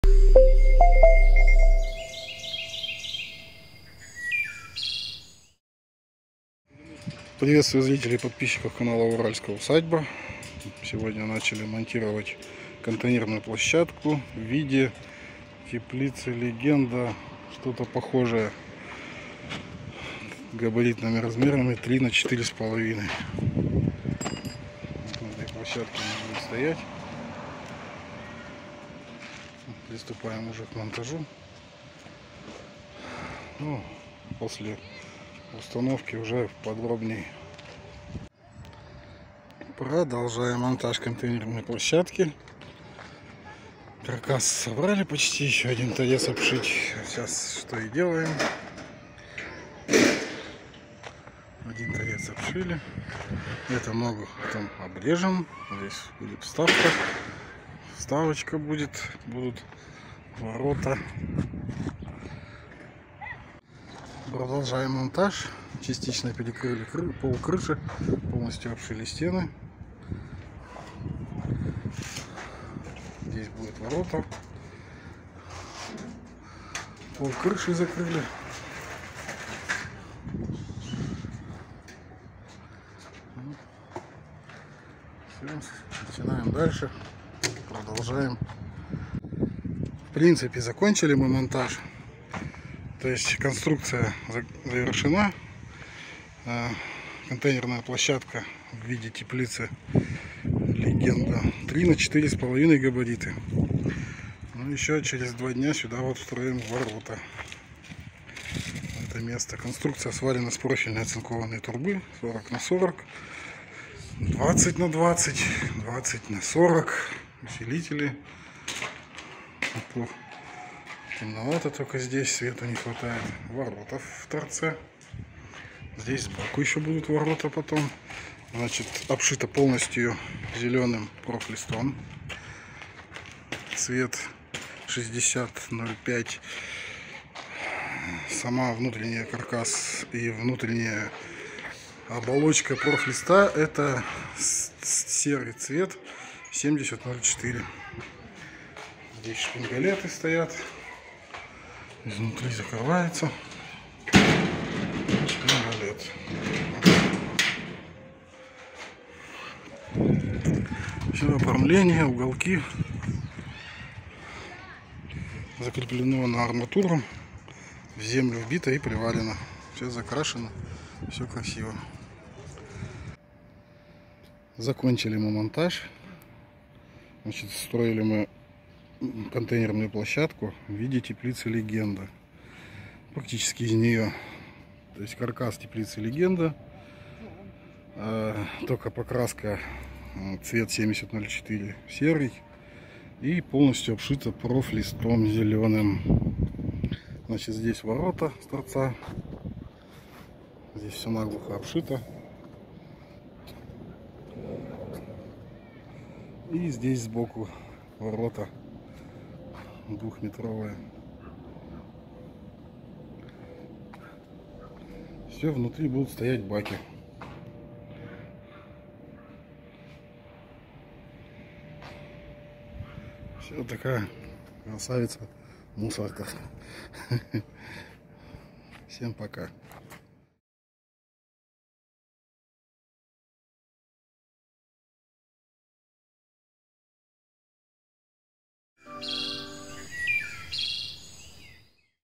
ЗАКРЫТАЯ ПЛОЩАДКА ДЛЯ СБОРА МУСОРА
С чего начинается порядок?

Когда мы организуем удобное и функциональное пространство, то встаёт вопрос о мусоре и отходах. Это важно для жилых участков, производственных площадок и любых местах пребывания людей. Так уж сложилось, что человек мусорит. Как складировать, хранить и вывозить мусор?
При правильной организации должен быть мусорный контейнер и площадка для него.

Все это можно заказать в компании «УРАЛЬСКАЯ УСАДЬБА»

Контейнерная площадка - это капитальная конструкция из оцинкованной трубы с порошково-полимерной окраской. Цвет 7004 серый.

Для изготовления каркаса  используется труба 20/40, 20/20 и 40/40, каркас монтируется на бетонную плиту или асфальтовую площадку. Конструкция  обшита профлистом, с8 цвет 6005.
Цвет сотового поликарбоната и профлиста, можно выбрать в нашем ассортименте.
Размер контейнерной площадки мы подберем под ваши запросы.
Для удобства, красоты и порядка в любом месте нужна контейнерная площадка с контейнером для мусора. Все это есть у нас.
 Обращайтесь и мы подберем конструкции  под ваши запросы!